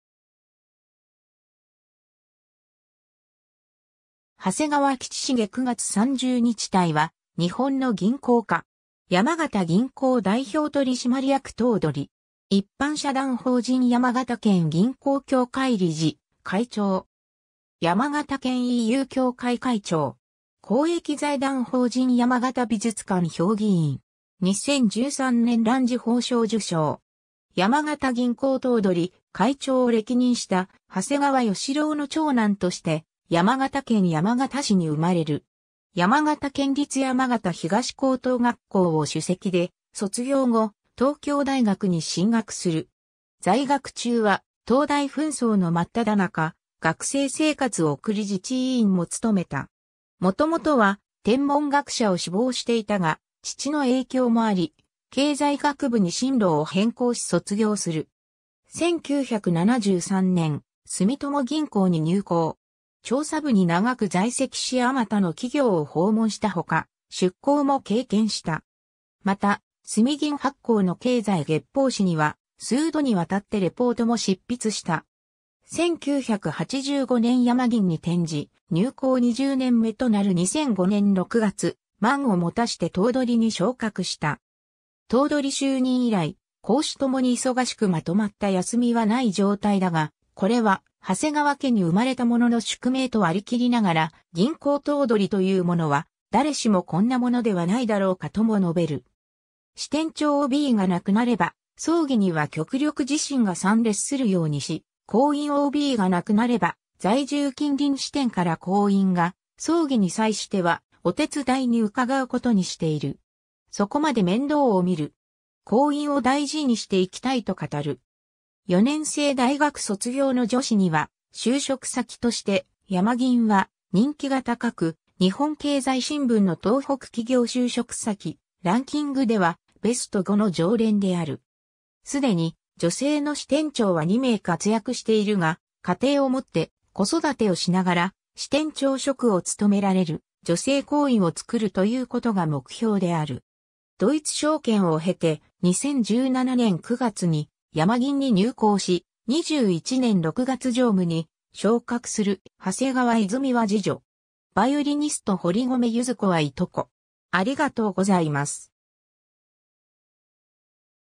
長谷川吉茂
長谷川 吉茂（はせがわ きちしげ、1949年（昭和24年）9月30日 - ）は、日本の銀行家。
山形銀行代表取締役頭取。
一般社団法人山形県銀行協会理事・会長。
山形県EU協会会長。
公益財団法人山形美術館評議員。
 2013年藍綬褒章受章。
 山形銀行頭取、会長を歴任した長谷川吉郎の長男として山形県山形市に生まれる。
 山形県立山形東高等学校を首席で卒業後、東京大学に進学する。
在学中は、東大紛争の真っ只中、学生生活を送り自治委員も務めた。
もともとは、天文学者を志望していたが、父の影響もあり経済学部に進路を変更し卒業する。
 1973年（昭和48年）、住友銀行（現・三井住友銀行）に入行。
調査部に長く在籍しあまたの企業を訪問したほか、出向も経験した。
また、住銀発行の経済月報誌には数度にわたってレポートも執筆した。
 1985年（昭和60年）山銀に転じ、入行20年目となる2005年（平成17年）6月、満を持して頭取に昇格した。
 頭取就任以来、公私共に忙しくまとまった休みはない状態だが、これは長谷川家に生まれたものの宿命と割り切りながら、「銀行頭取というものは、誰しもこんなものではないだろうか」とも述べる。
支店長OBが亡くなれば、葬儀には極力自身が参列するようにし、行員OBが亡くなれば、在住近隣支店から行員が、葬儀に際してはお手伝いに伺うことにしている。
そこまで面倒をみる。
行員を大事にしていきたいと語る。
4年制大学卒業の女子には、就職先として山銀は、人気が高く、日本経済新聞の東北企業就職先ランキングでは、ベスト5の常連である。
すでに、女性の支店長は2名活躍しているが「家庭をもって、子育てをしながら、支店長職を勤められる女性行員をつくる」ということが目標である。
 ドイツ証券を経て、2017年（平成29年）9月に山銀に入行し、21年6月常務に昇格する長谷川泉は次女。
バイオリニスト堀米ゆず子はいとこ。